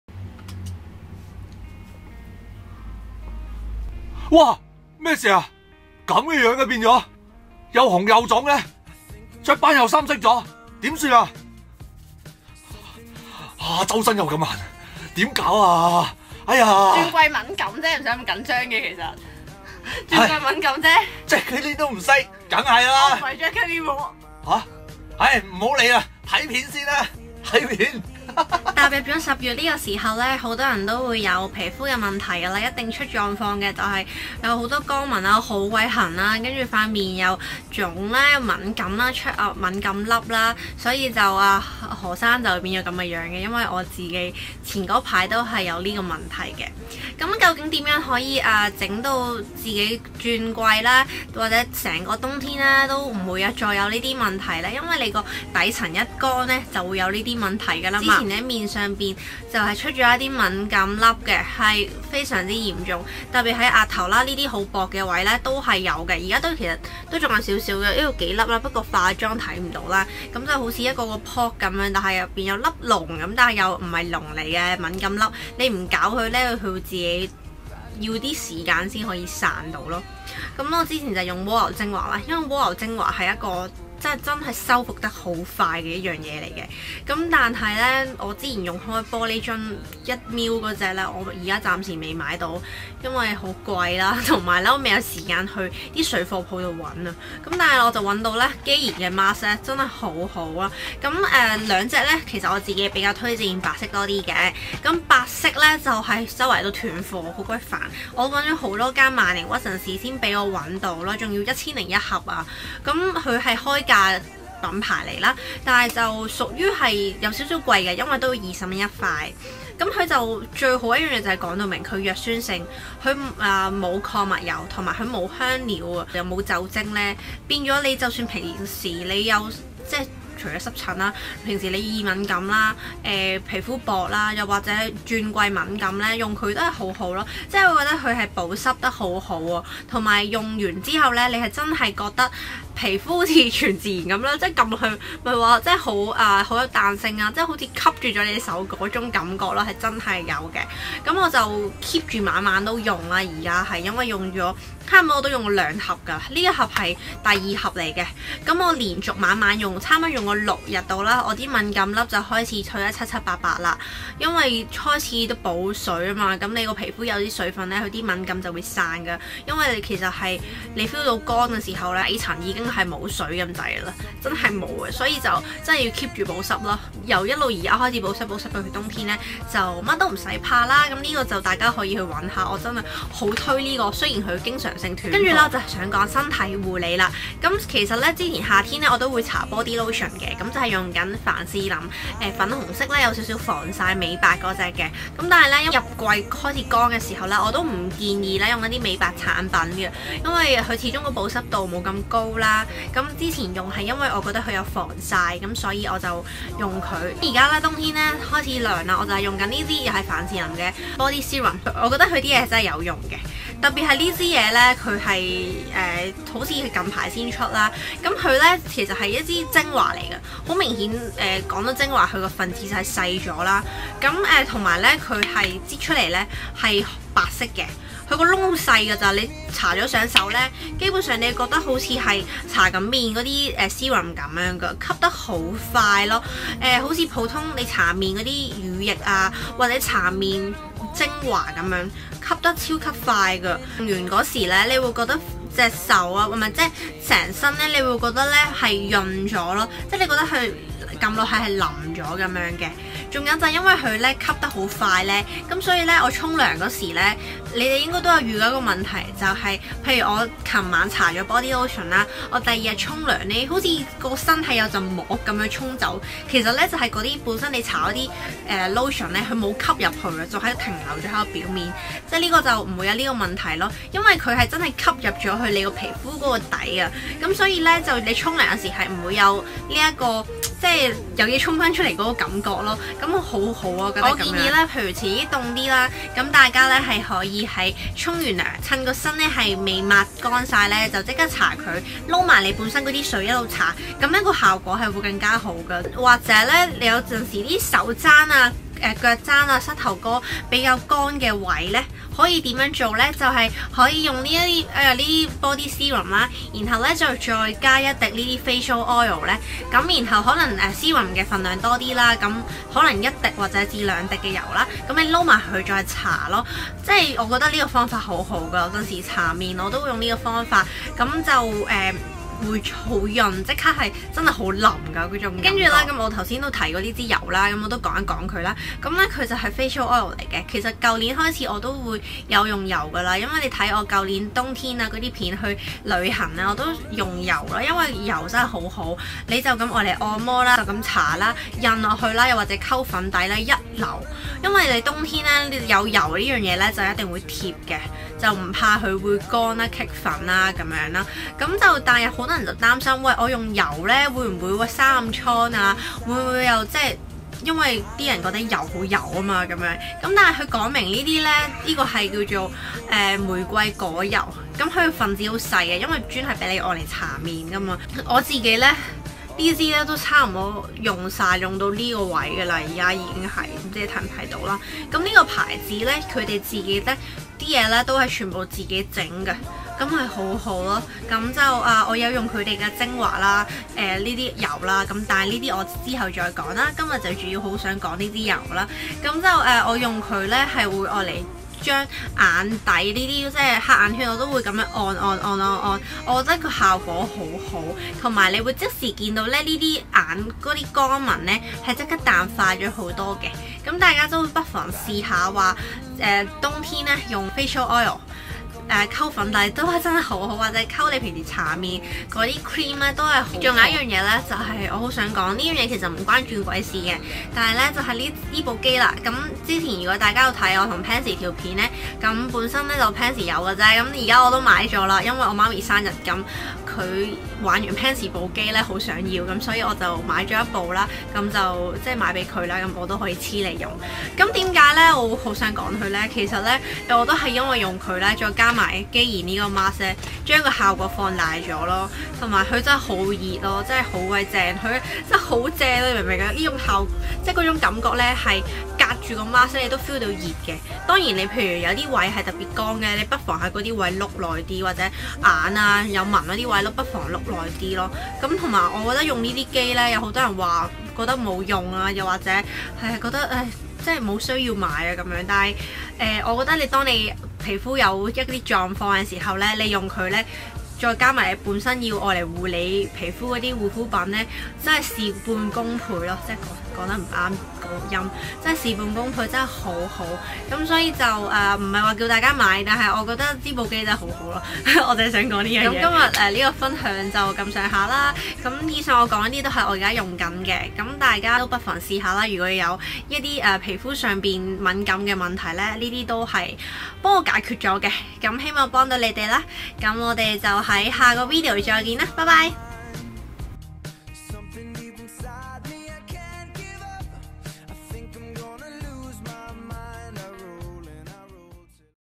Cherrie's Daily~ 轉季敏感大作戰
The products mentioned in the video:
1. 肌研 極潤系列 極潤保濕面膜
2.Vaseline Total Moisture Nourishing serum
3.Start from natural Rose Hip Seed Oil
4.Glycel 42°C Mobile Skin therapist

Facebook fans page |cherrie1204|
XOXO